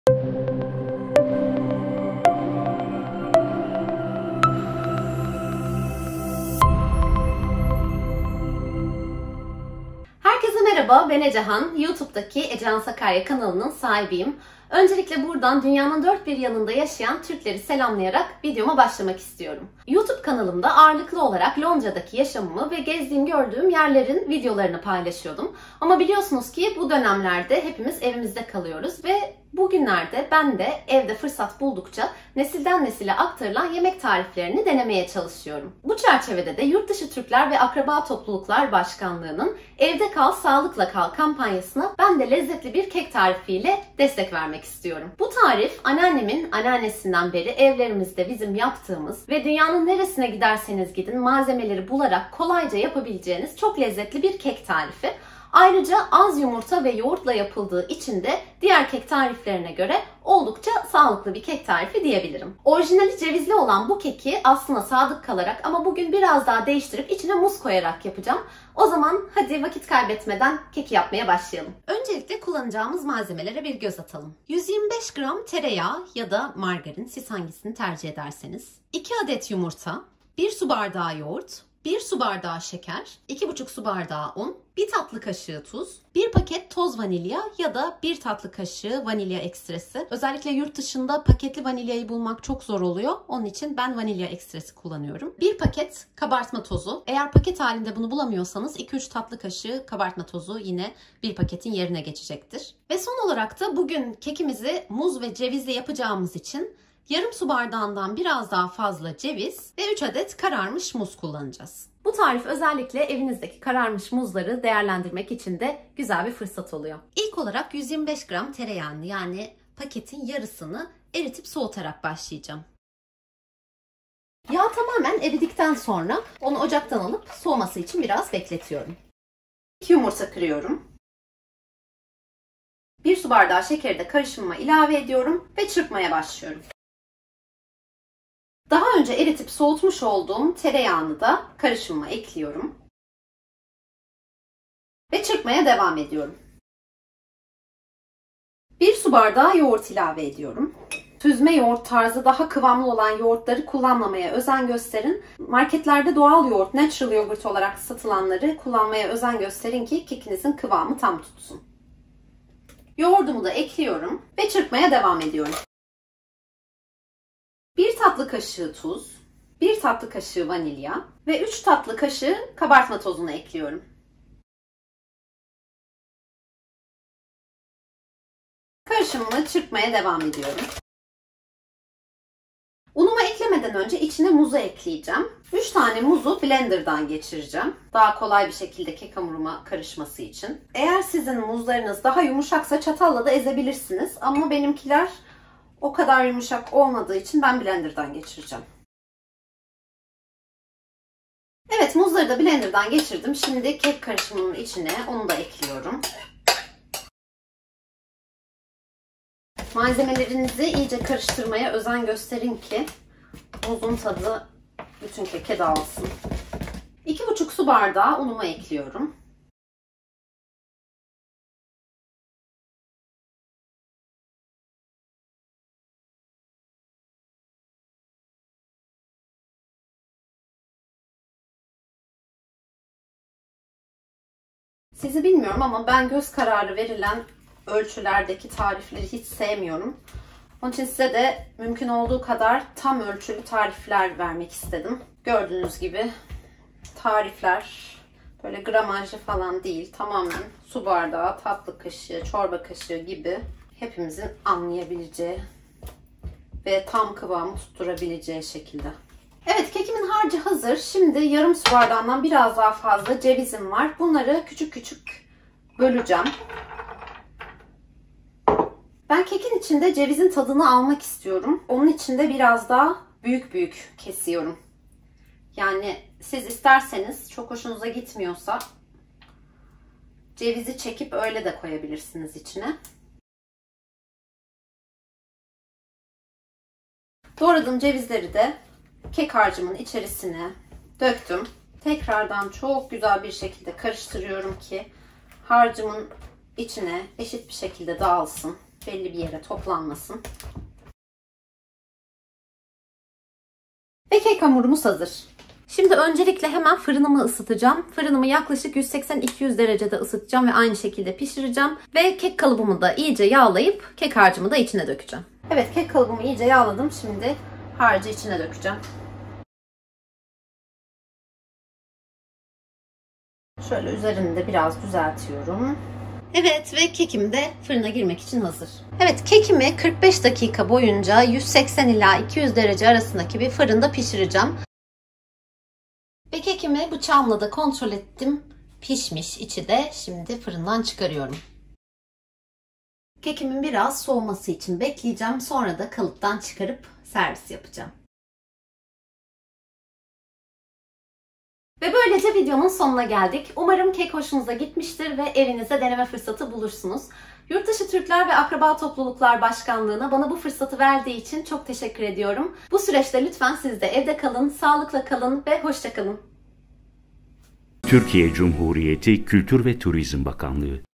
Muzlu Kek - Ecehan Sakarya Haner - İngiltere
Diasporadan tarifler bölümüzde bugün İngiltere'ye konuk oluyoruz

Londra'dan Ecehan Sakarya Haner bizlerle nesilden nesile geçen muzlu kek tarifini paylaşarak EvdeKal mesajı veriyor.